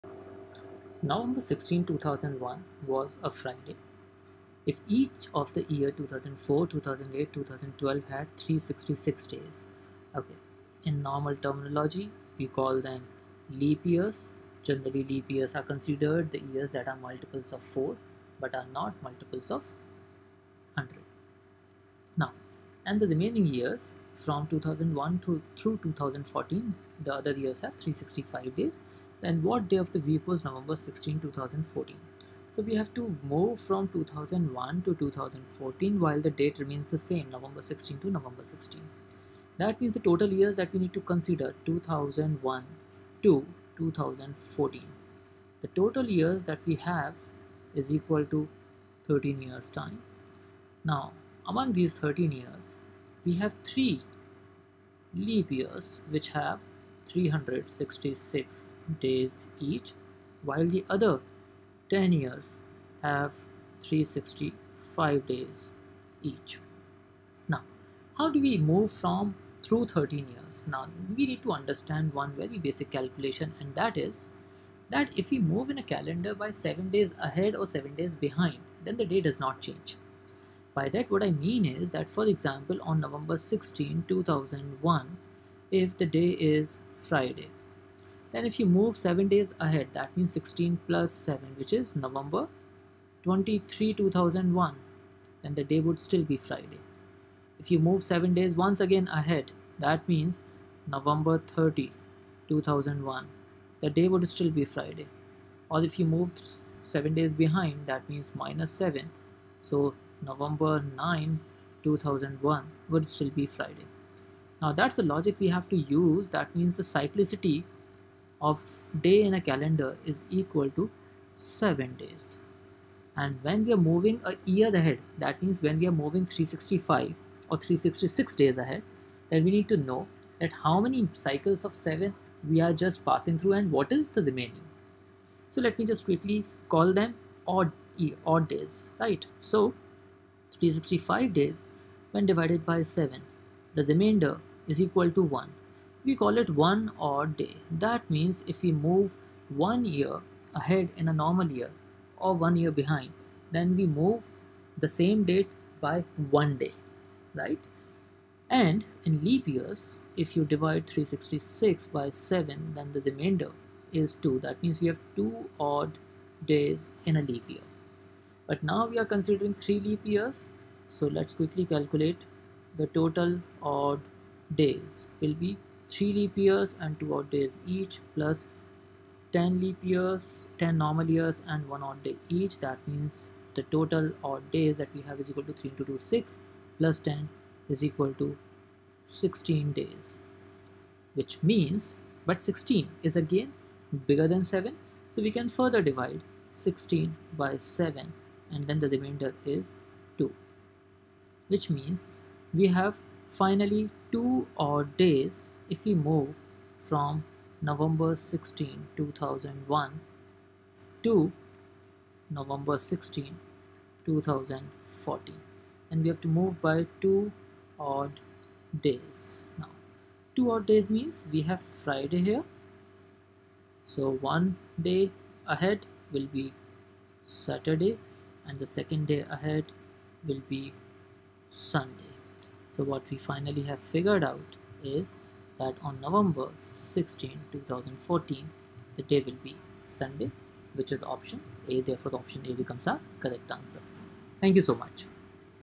OG21 PS 234 Hard Calander cyclicity
November 16, 2001, was a Friday. If each of the years 2004, 2008, and 2012 had 366 days, and the remaining years from 2001 through 2014 had 365 days, what day of the week was November 16, 2014 ?

A. Sunday
B. Monday
C. Tuesday
D. Wednesday
E. Thursday